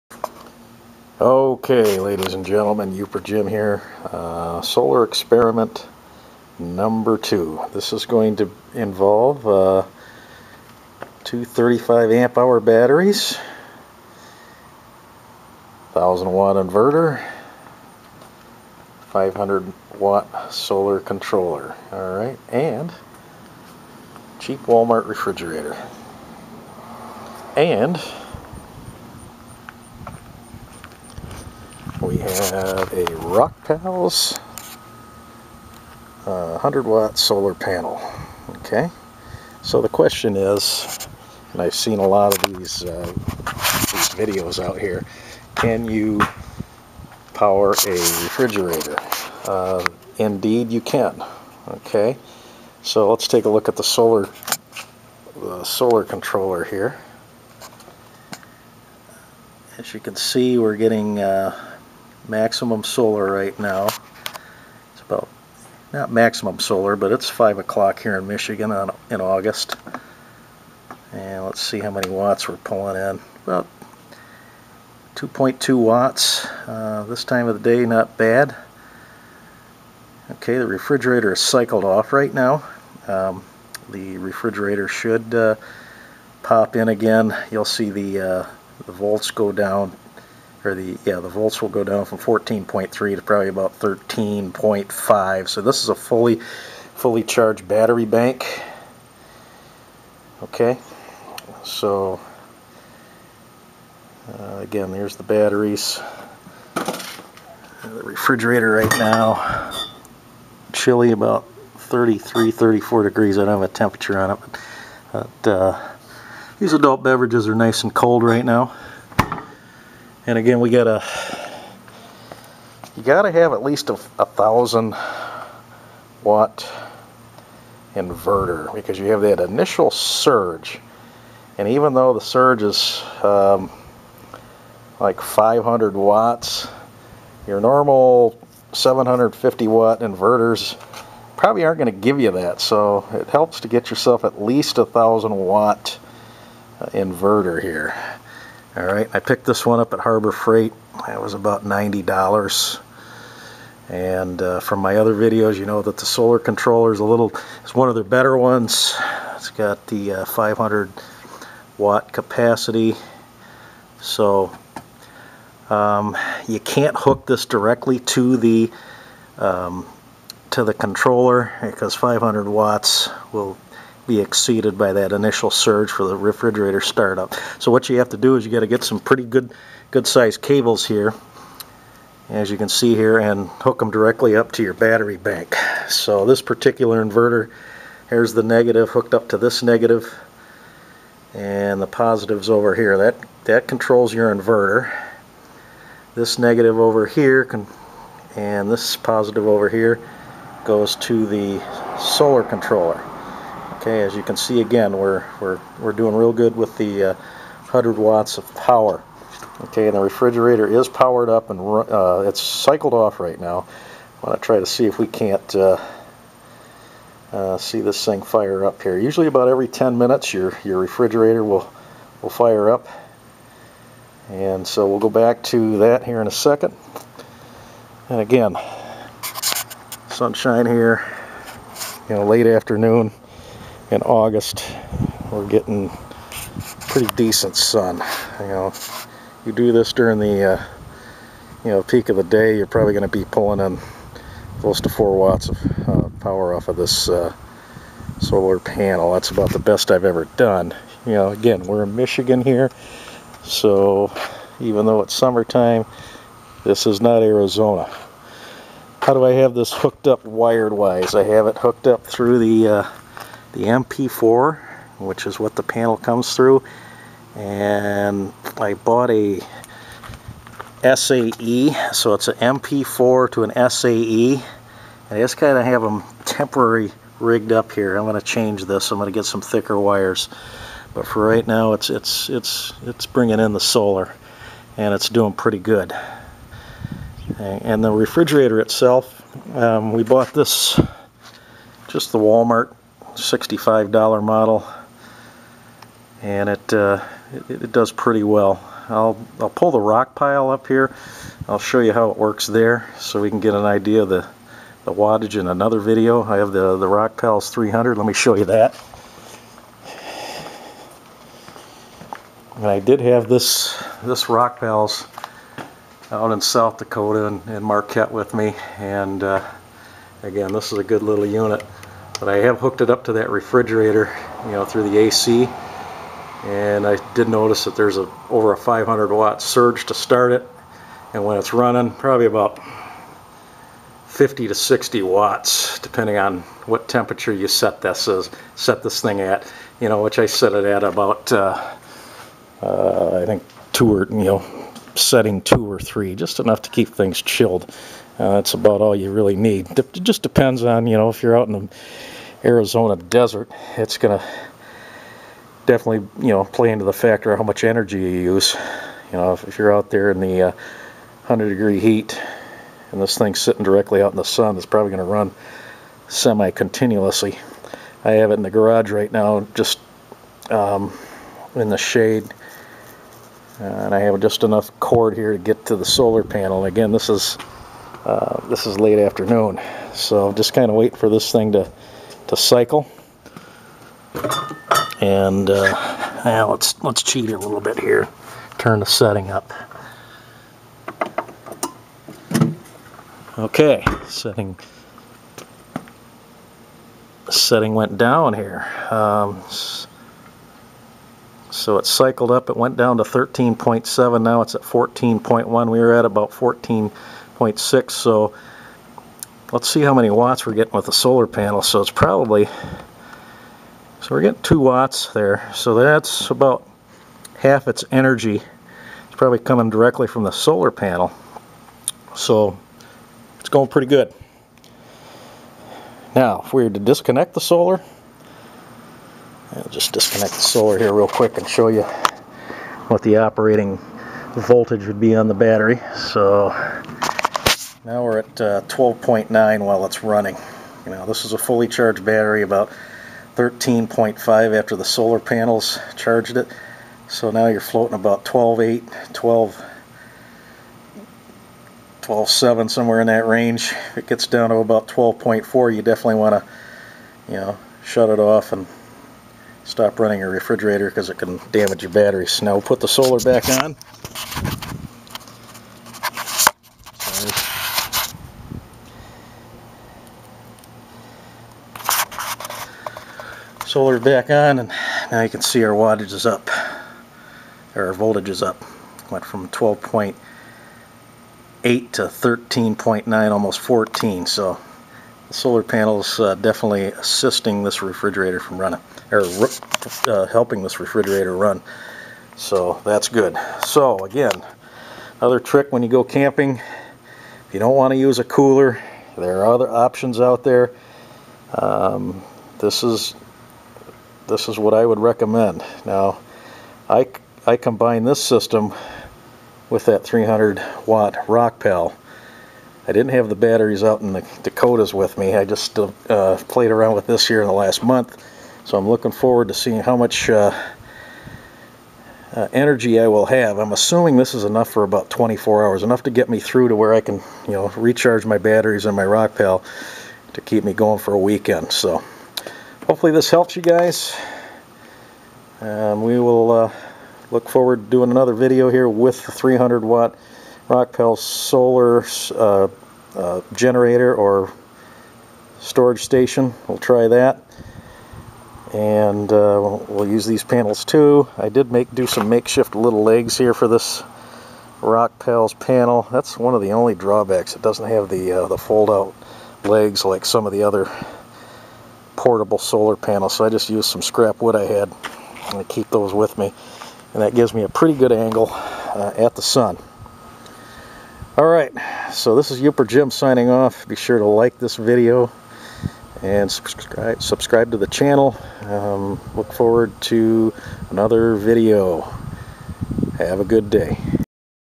Solar Powered Mini Fridge
An experiment using Rockpals 100w solar panel and Harbor Freight 1000 watt Inverter.